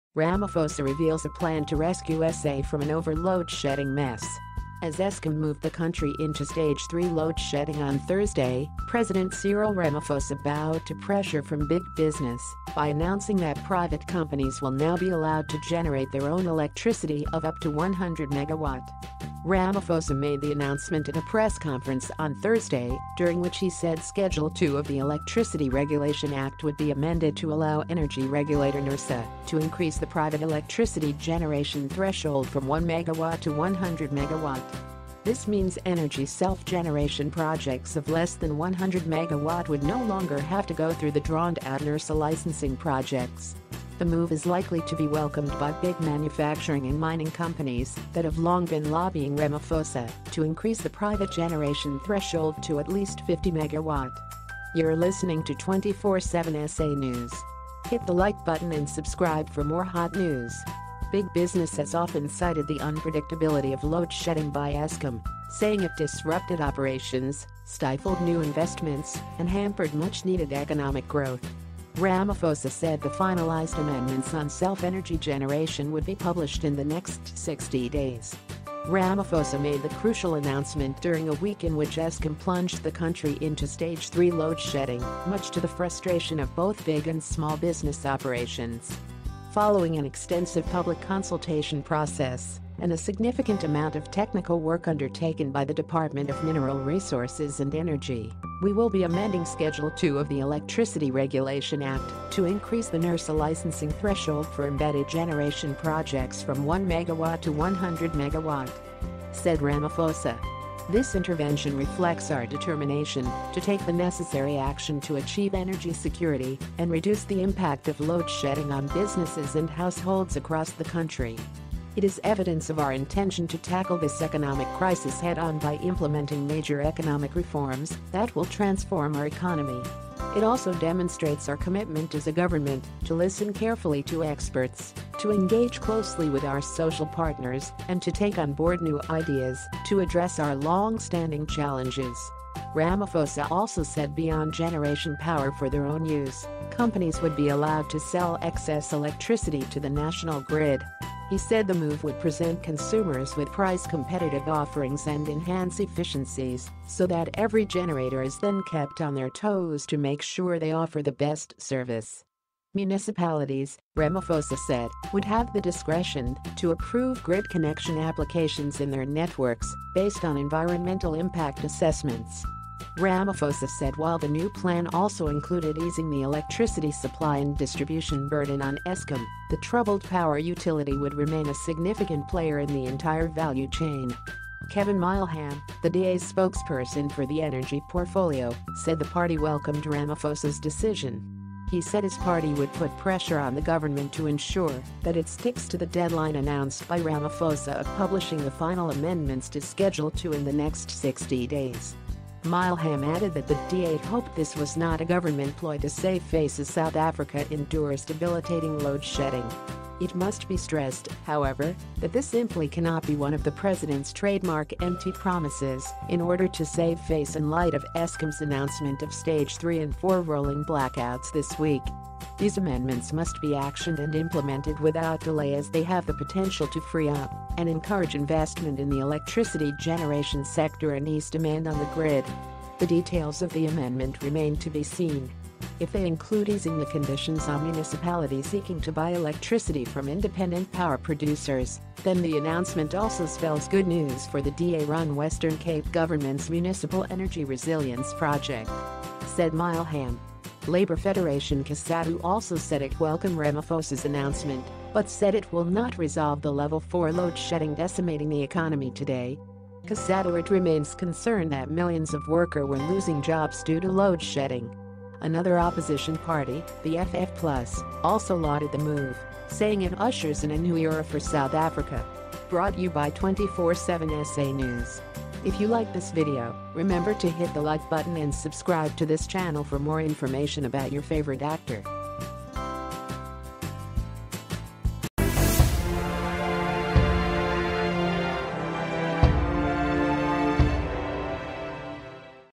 President Cyril Ramaphosa reveals a plan to rescue SA from an over-load-shedding mess.
As Eskom moved the country into stage three load-shedding on Thursday, President Cyril Ramaphosa bowed to pressure from big business by announcing that private companies will now be allowed to generate their own electricity of up to 100 MW.
Ramaphosa made the announcement at a press conference on Thursday, during which he said schedule 2 of the Electricity Regulation Act would be amended to allow energy regulator Nersa to increase the private electricity generation threshold from 1 MW to 100 MW.